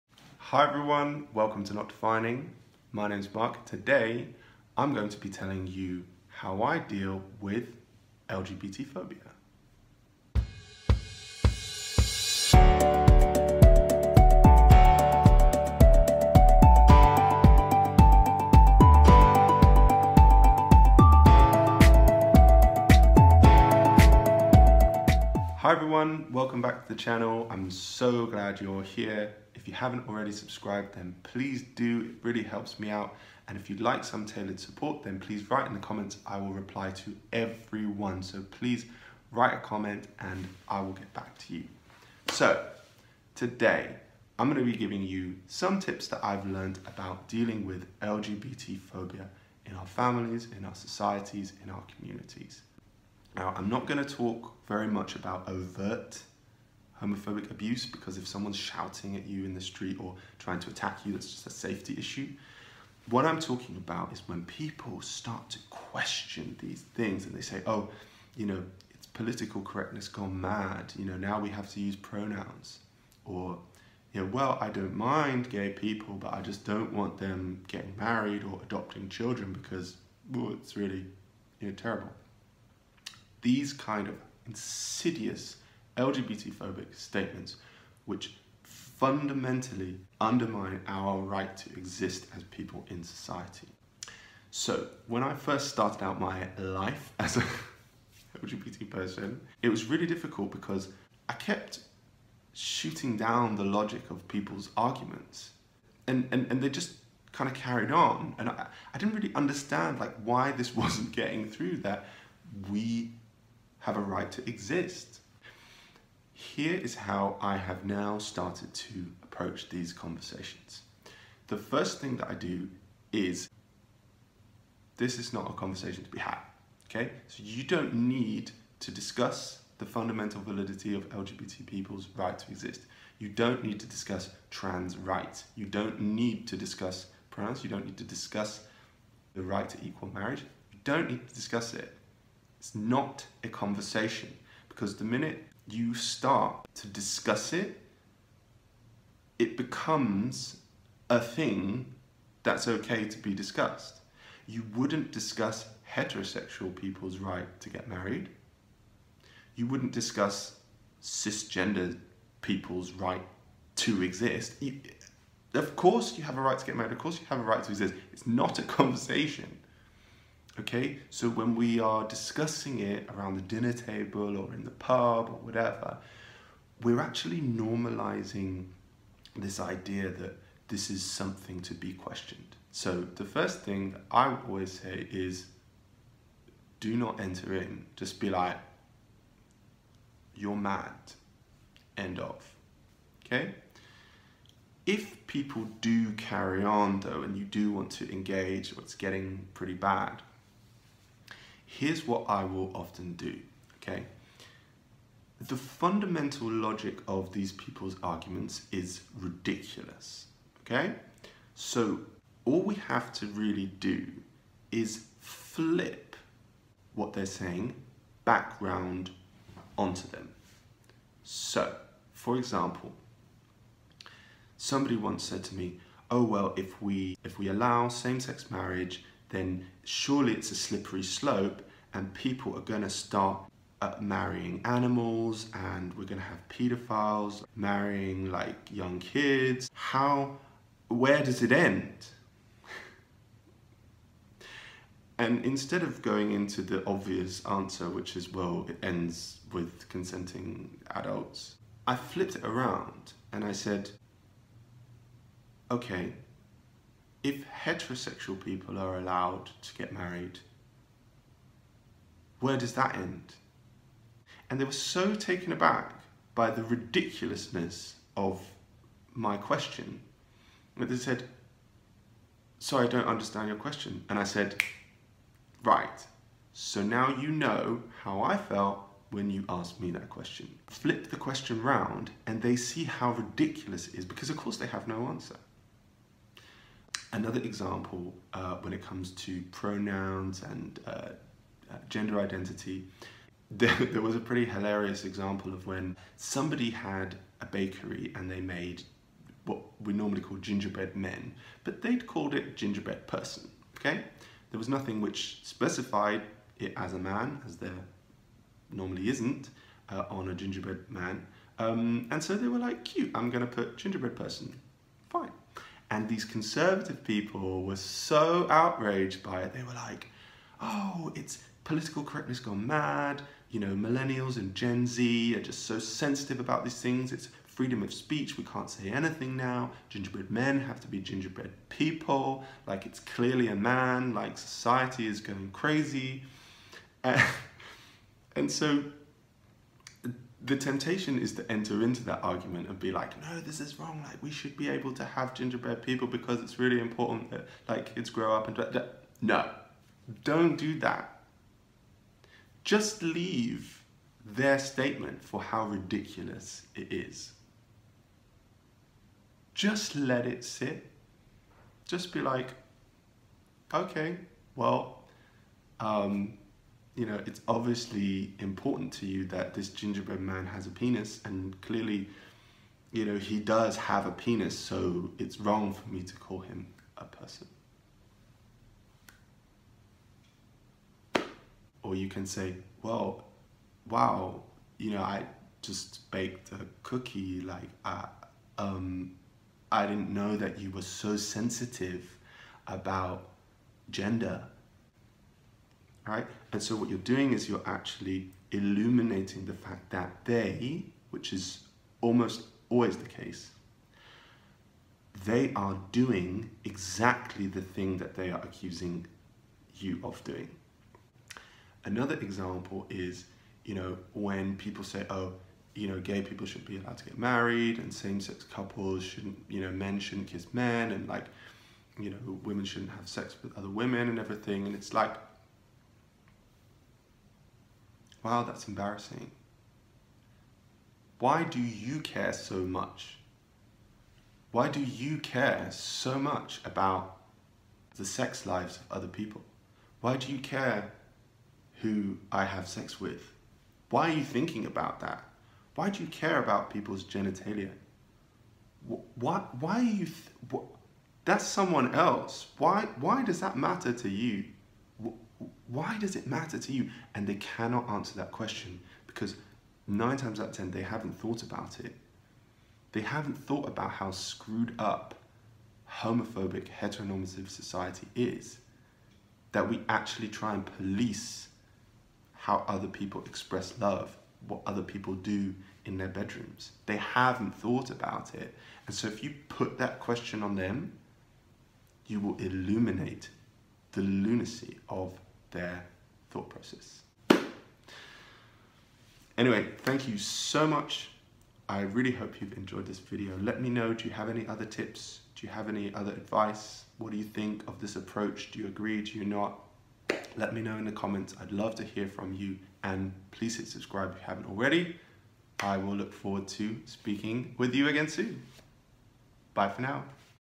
Homophobia | method for stopping LGBT-phobia (works every time)
Ever wondered how to deal with homophobic remarks? Ever felt frustrated that you can’t seem to explain LGBT rights to homophobic people?

In this video I share a tried and tested method to shut down homophobia every time. It focuses on self confidence and standing your ground.

This is an incredible method for stopping homophobia every single time. No matter where you are or who you are, it really works. Never again get frustrated with homophobic remarks or comments. You can even use this to deal with homophobic families or colleagues.

Don't let homophobia, queerphobia or transphobia win. Stop it in your tracks with this simple method.

Check us out @notdefining on Twitter Instagram and TikTok. Also check out The Queer Spectrum podcast on all major podcast platforms.